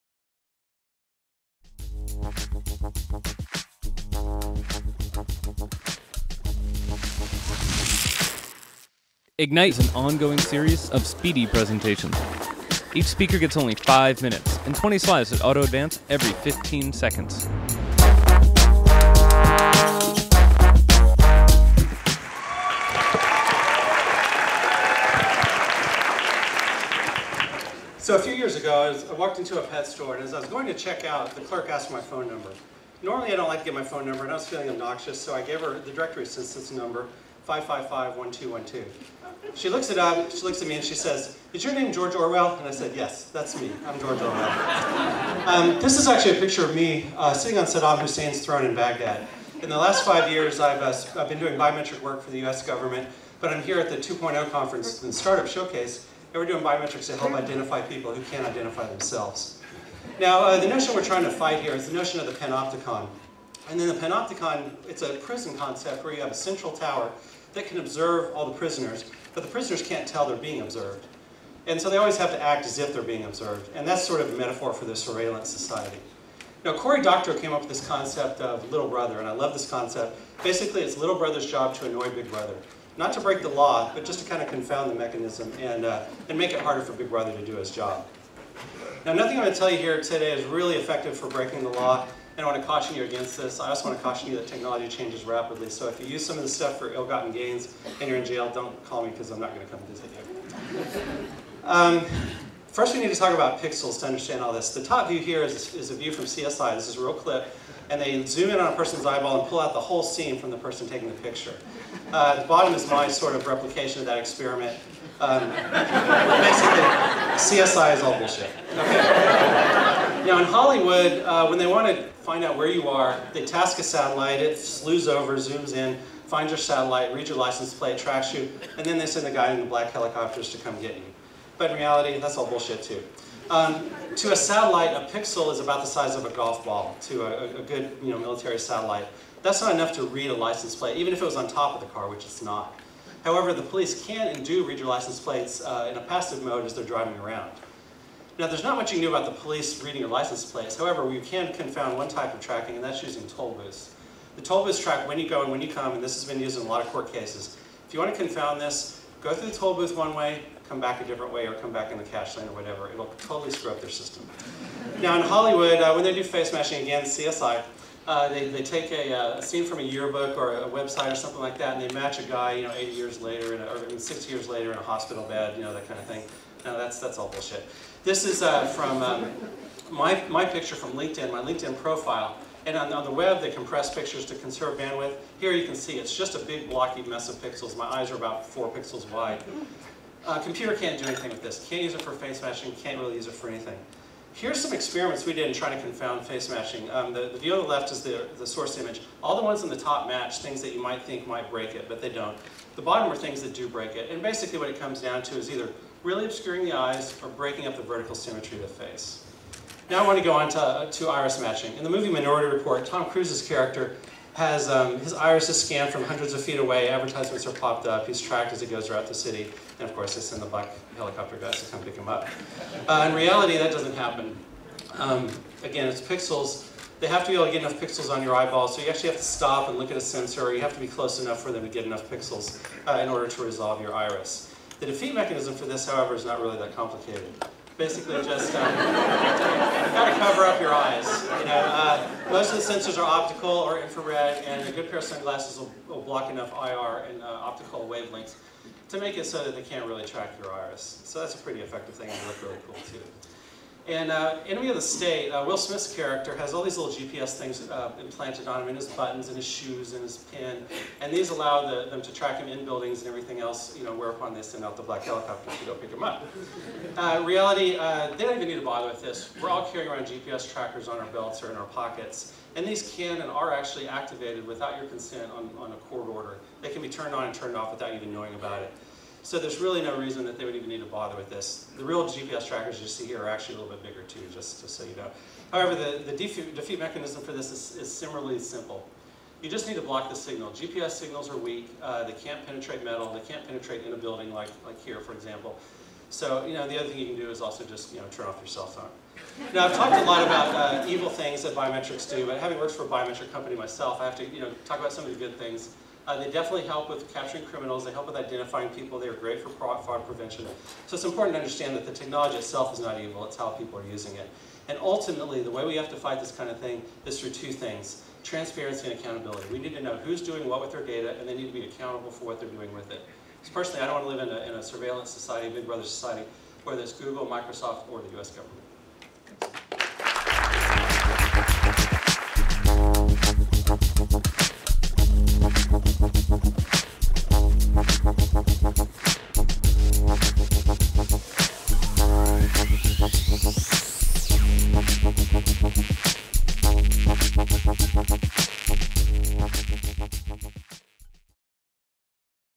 Defeating Big Brother by Dr. Alex Kilpatrick
Cory Doctorow posited that "Little Brother's job is to mess with Big Brother". To do that you need to know what tools Big Brother is using. In the case of biometrics there's a lot of misinformation out there. For example Hollywood shows people being identified by mere pixels or artificially aged yearbook photos.

Dr. Alex Kilpatrick has worked on biometrics for the past couple of years. His experiments show that Hollywood is taking creative liberties when writing their shows. Alex shows us what does work and how you can thwart Big Brother's systems that are monitoring you. Biometrics themselves aren't bad or evil. They are used to prevent any number of crimes, but knowing what's possible and what isn't will lead to a more transparent society.